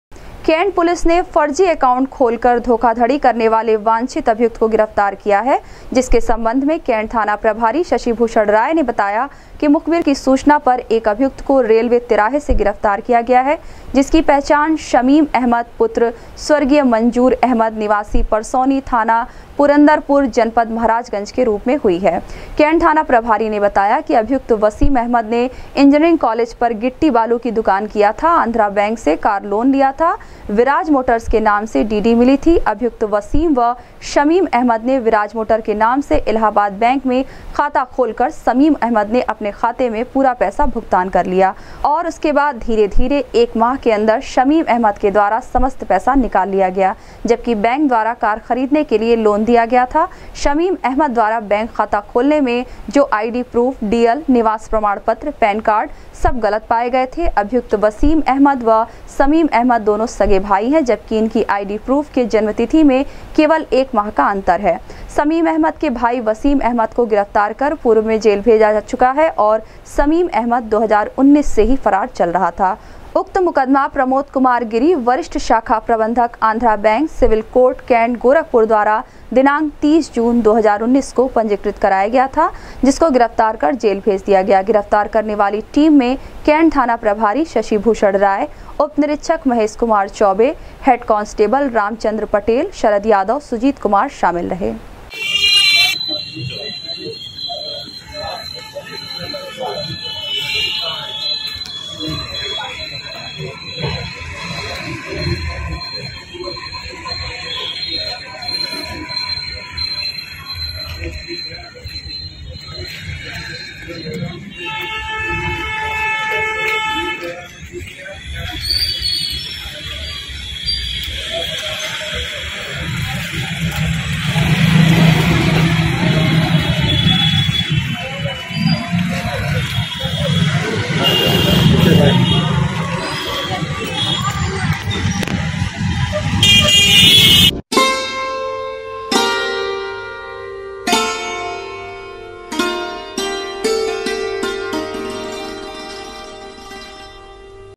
फर्जी एकाउण्ट खोलकर धोखाधड़ी करने वाले वांछित एक अभियुक्त शमीम अहमद को कैंट पुलिस ने किया गिरफ्तार
फर्जी एकाउण्ट खोलकर धोखाधड़ी करने वाले वांछित एक अभियुक्त शमीम अहमद को कैंट पुलिस ने किया गिरफ्तार,भेजा जेल